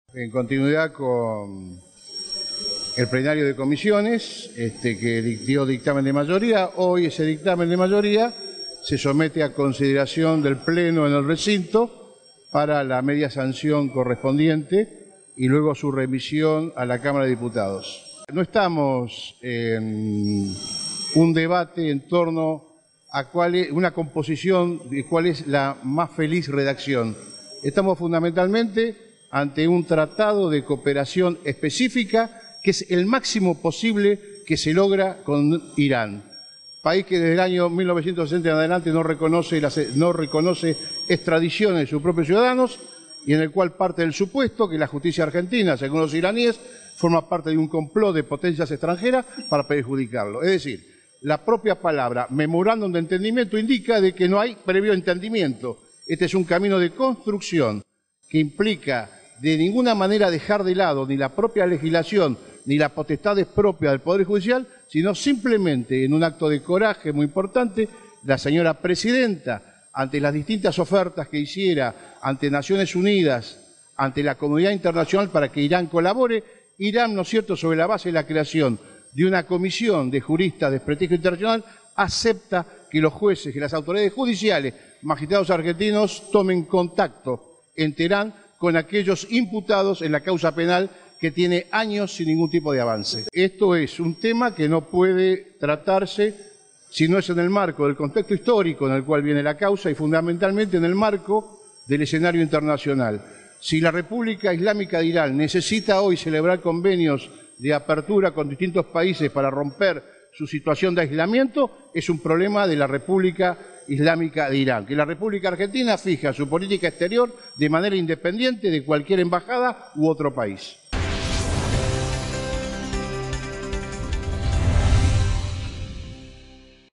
El senado comenzó a debatir el acuerdo con Irán por la causa AMIA
El Senado comenzó cerca del mediodía de hoy, la sesión en la que se debatirá el acuerdo que Argentina suscribió con Irán para interrogar en ese país a ciudadanos iraníes acusados del atentado a la AMIA.